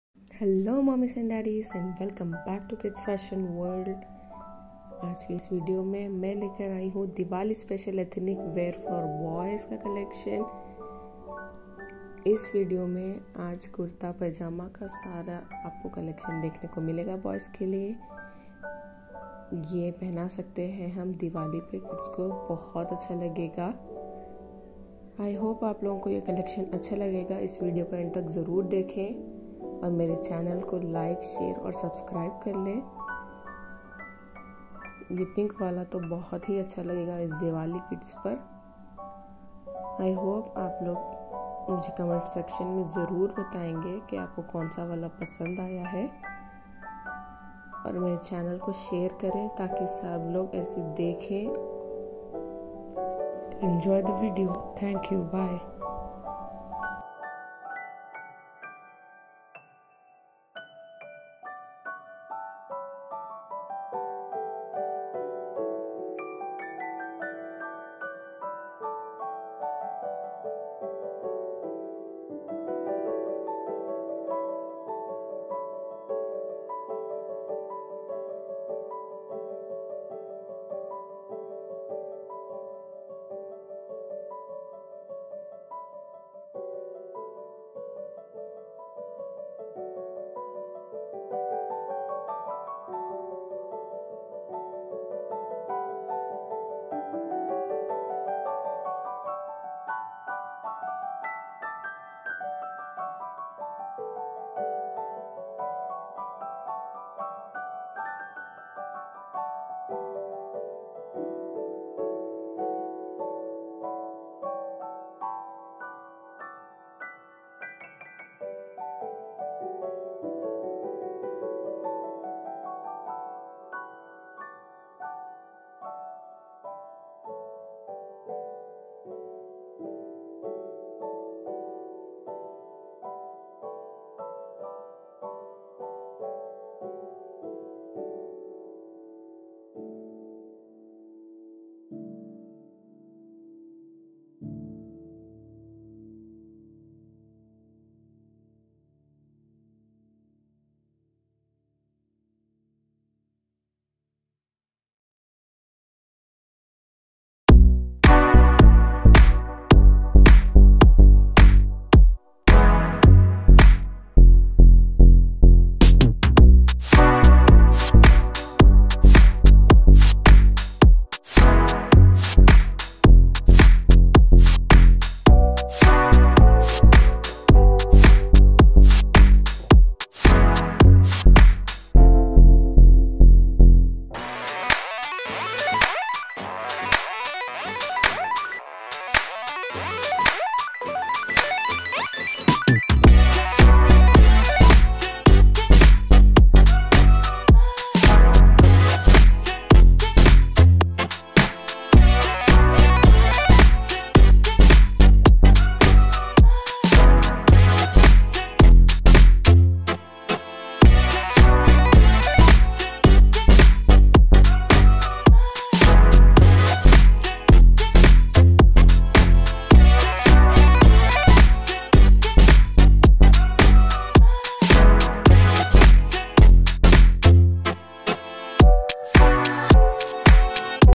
Diwali Special Ethnic Wear For Boys...
Beautiful Diwali special ethnic wear collection for boys. kurta pajama sets for boys